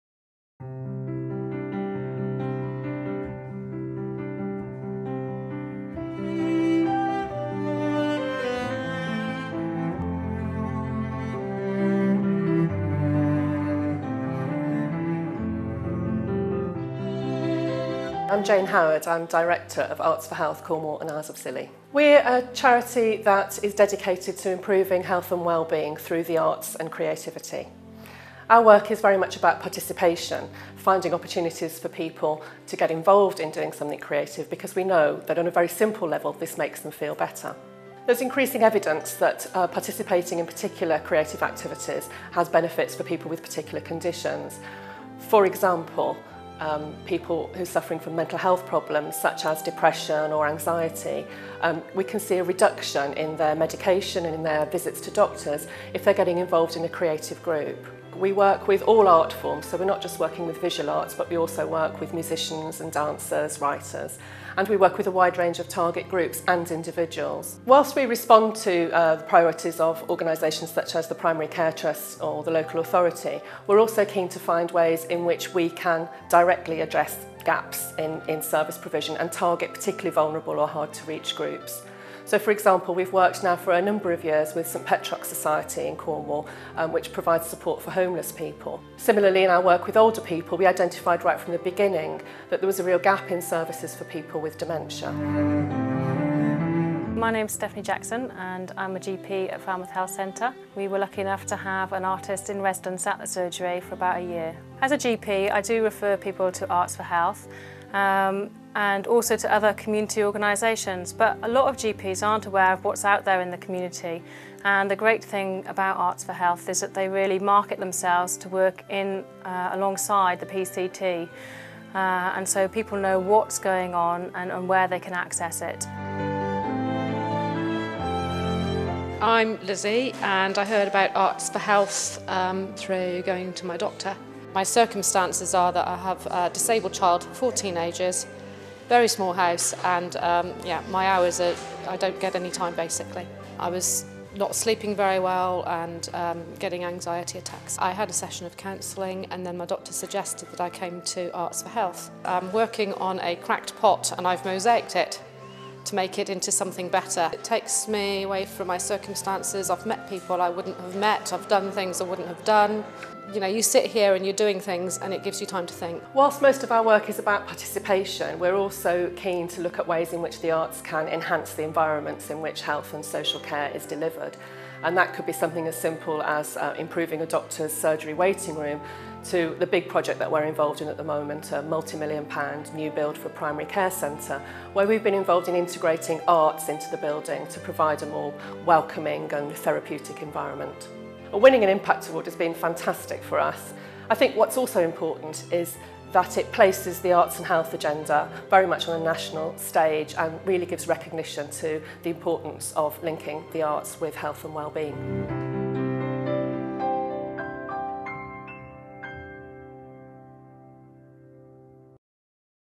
GSK IMPACT Awards 2010 winners: Arts for Health Cornwall and Isles of Scilly
Video produced in May 2010. Arts for Health Cornwall and Isles of Scilly aim to improve health and wellbeing through creativity. They were one of The King's Fund and GSK IMPACT Award winners in 2010.

Arts for Health Cornwall and Isles of Scilly was a winner of The King's Fund 2010 GSK IMPACT Awards and received an unrestricted donation of £25,000 plus places for two people to take part in the GSK IMPACT Awards Development Network course.